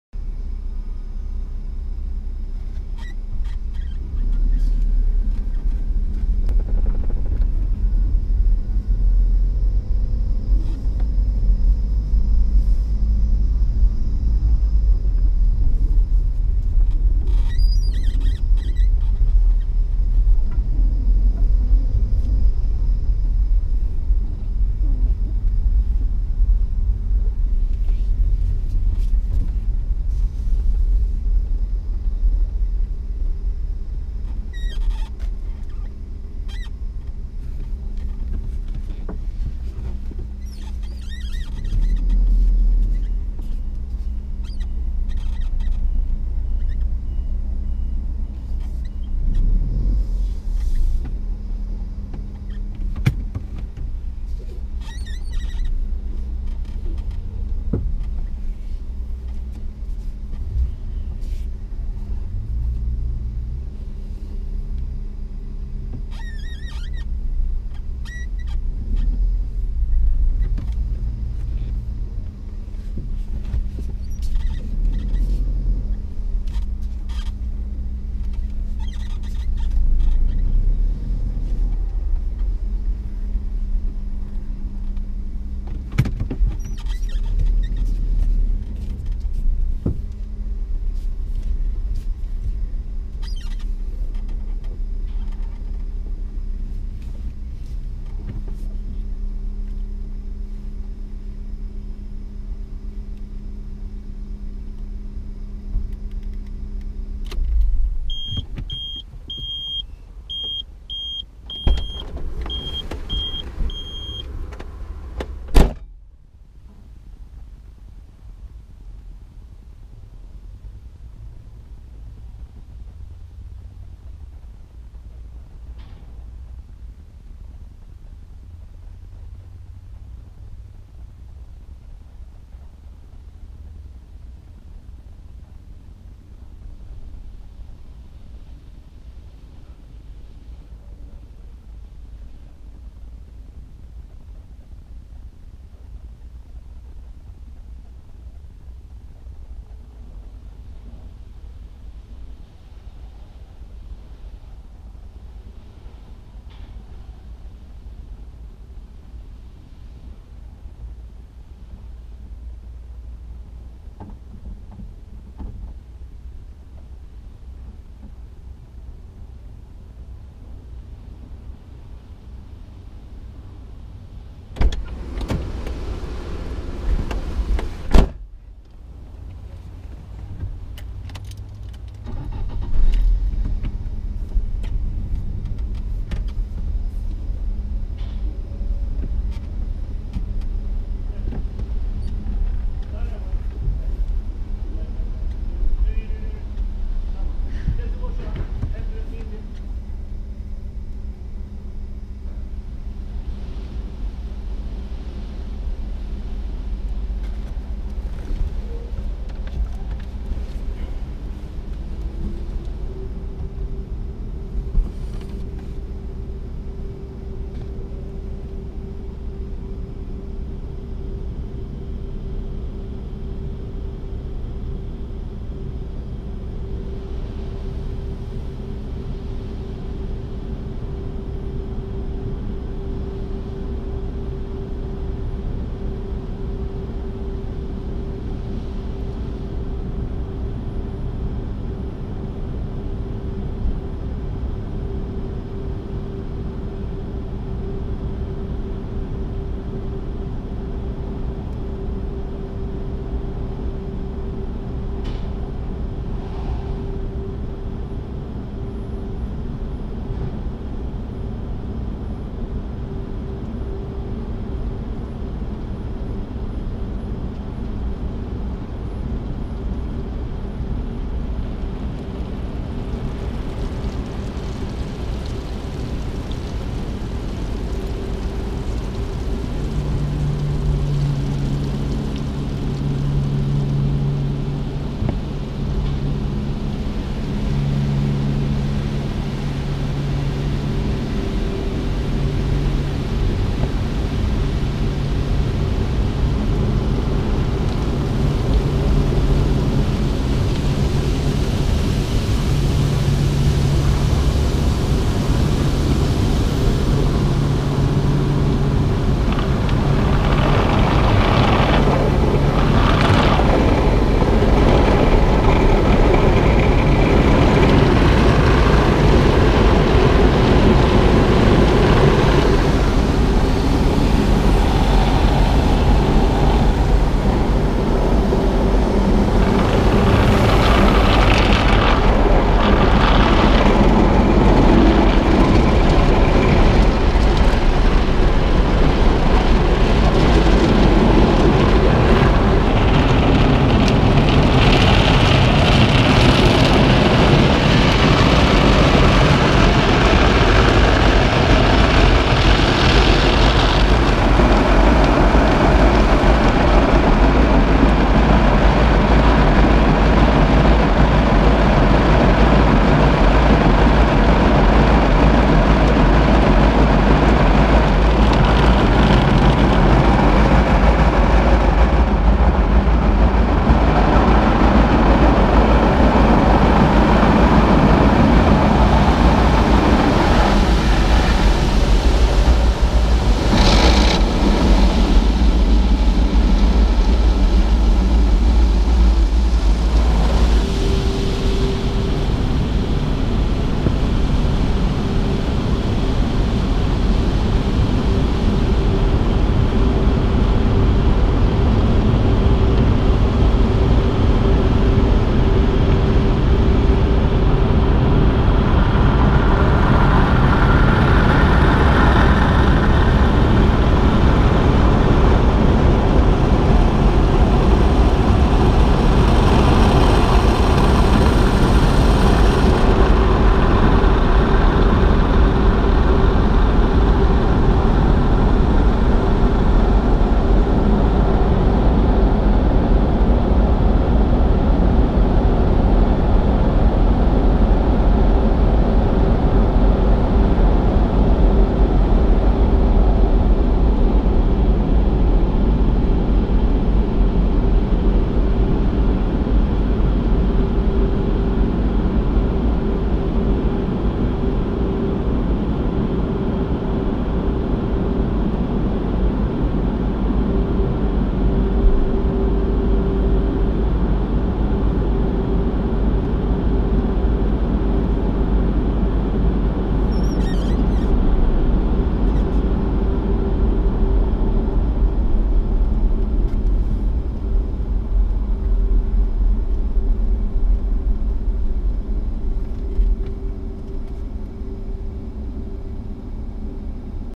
BP NİN YENİ NESİL ARAÇ YIKAMA SİSTEMİ
BP istasyonlarında sabunlu köpüklü cilalı lastik yıkamalı ve kurutmalı araç yıkama sistemini izleyeceksiniz. Bu hizmetin ücreti 8 tl.